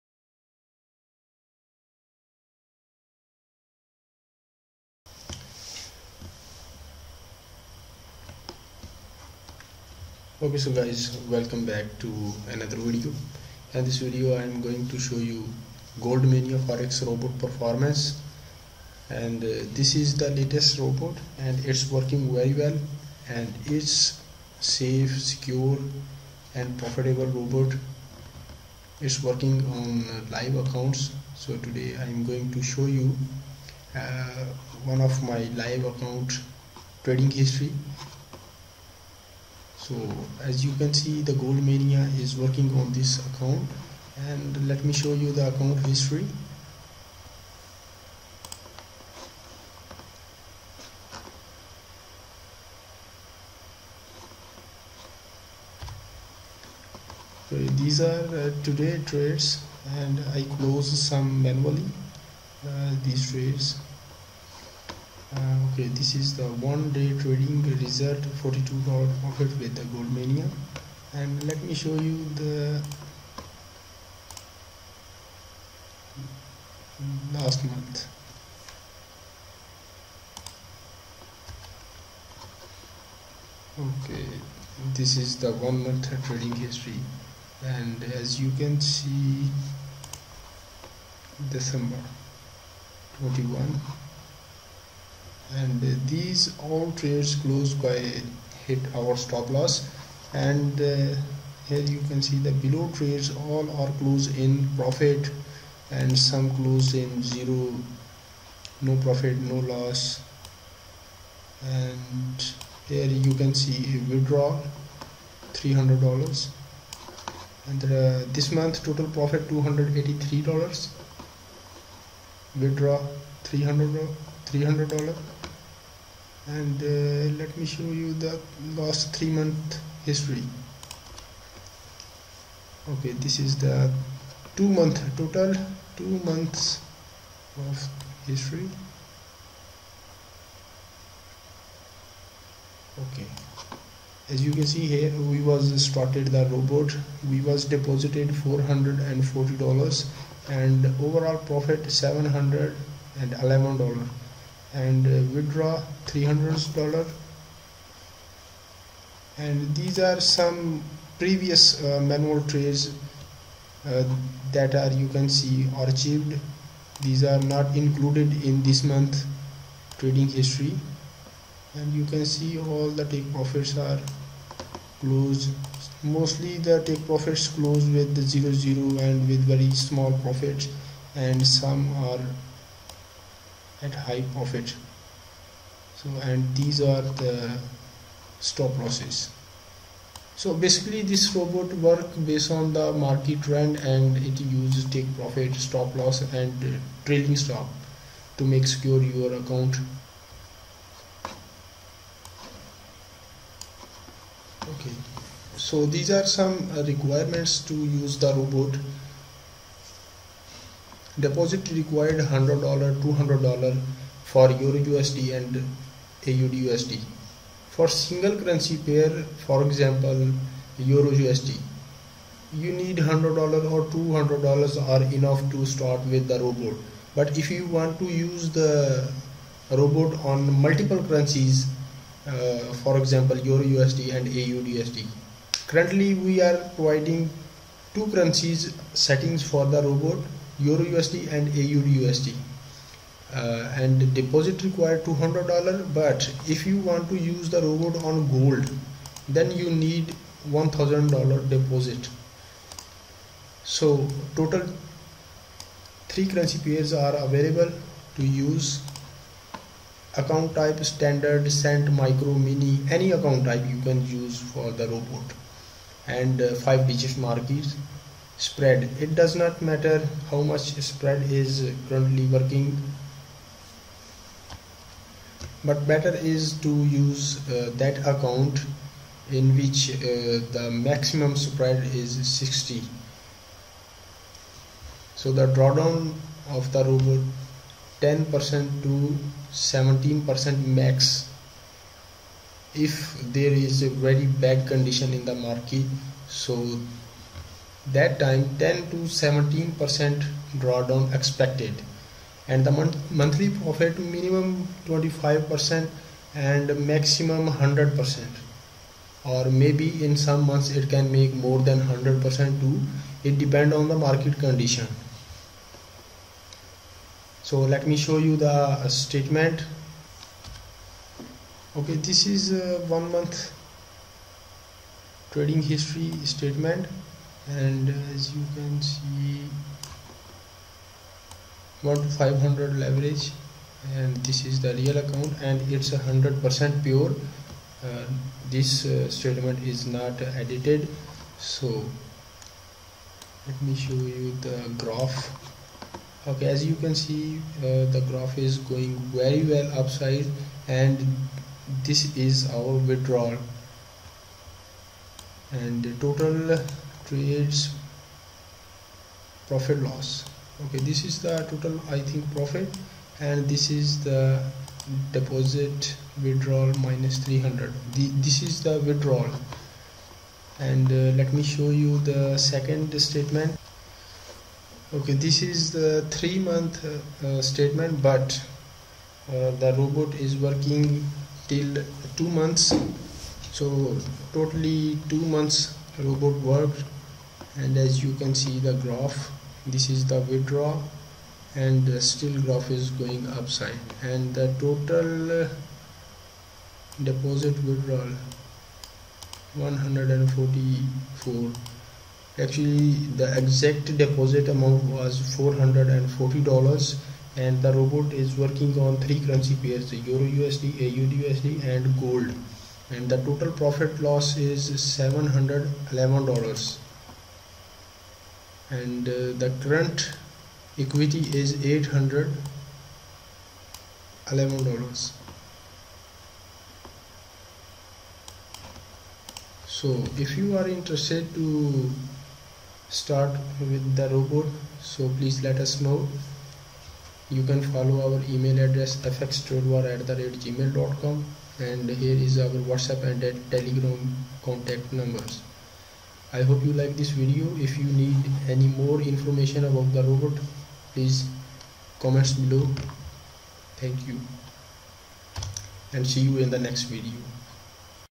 Goldmania Best Forex Robot 2023 forex Automated Profit Live Account: Low Draw-down
New Year Best Forex Robot with Live Account Performance. Live Trading with SL/TP Protected Forex Robot.

Deposit Required : 100$
 EURUSD,AUDUSD for 2 Pairs = 200$
XAU-USDGOLD = 1000$

Account Type: Standard, Cent , Micro Mini, Any Account better is to Avoid ECN Account Type.
Market : 5 Digits
Spread: Max 60  no issue with spread but Lower spreads are much better.
Draw-Down : 10 to 17% max in worst Case scenario
Monthly Profit: 25% to 100% depend on the Market Condition

if you are interested and want to get the robot please let us know.